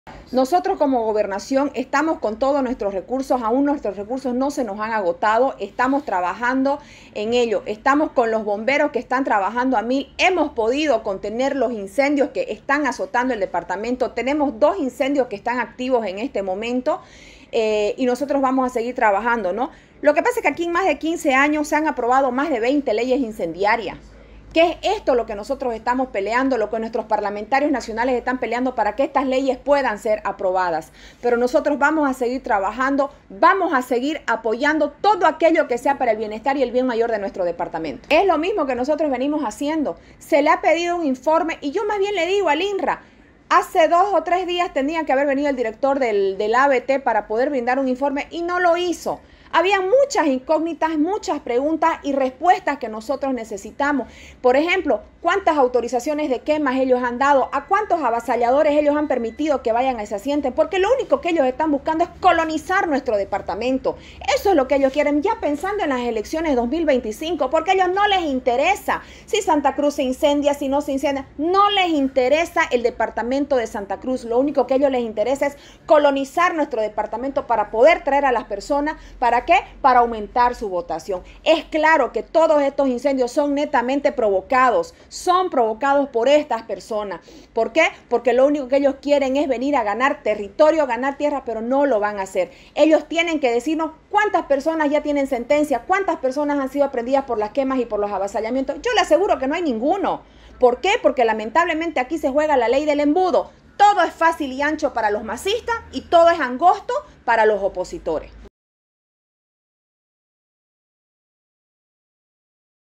García: “La Gobernación está atendiendo la emergencia por los incendios, actualmente hay dos activos
*García: “La Gobernación está atendiendo la emergencia por los incendios, actualmente hay dos activos en Santa Cruz”*

*Santa Cruz, 22 de noviembre de 2023.-* Tras la realización de la Asamblea de la Cruceñidad ayer, la asambleísta departamental Keila García, expresó que la Gobernación cuenta con recursos, aún no se han agotado, estamos trabajando en ello. Esto se refiere a las decisiones tomadas en la Asamblea de la Cruceñidad, que incluyen la declaración de estado de desastre debido a los incendios.

“Hemos podido contener los incendios que están azotando el departamento, en este momento tenemos dos incendios que están activos. García señaló que en más de 15 años se han aprobado más de 20 leyes incendiarias. “Esto es por lo que los parlamentarios nacionales están peleando, por lo que nosotros peleamos, para que estas leyes puede ser aprobadas. Vamos a seguir trabajando, apoyando todo aquello que sea para el bienestar del bien mayor de nuestro departamento”.

Asimismo, señaló que hace 48 horas se solicitó un reporte al Director de la Autoridad de Bosques y Tierras (ABT) para que proporcionara un informe, pero hasta el momento no ha cumplido con dicha solicitud. “Habían muchas incógnitas, muchas preguntas y respuestas que nosotros necesitamos, como ser: ¿cuántas autorizaciones de quemas han dado? a ¿a cuántos avasalladores han permitido que vayan y se asienten?. Lo único que ellos quieren es colonizar nuestro  departamento, pensando en las elecciones 2025, traer a estas personas para aumentar su votación. A ellos no les interesa  si Santa Cruz se incendia  o no. A ellos no les interesa Santa Cruz”.

“Es claro que todos estos incendios son netamente provocados por estas personas, porque lo que ellos quieren es venir a ganar territorio, tierras. Ellos tienen que decirnos ¿cuántas personas tienen sentencias?, ¿cuántas han sido aprendidas por las quemas y avasallamiento?, les aseguro que ninguno. Aquí se juega la ley del embudo todo es fácil y ancho para los masistas, y todo es angosto para los opositores”, expresó García.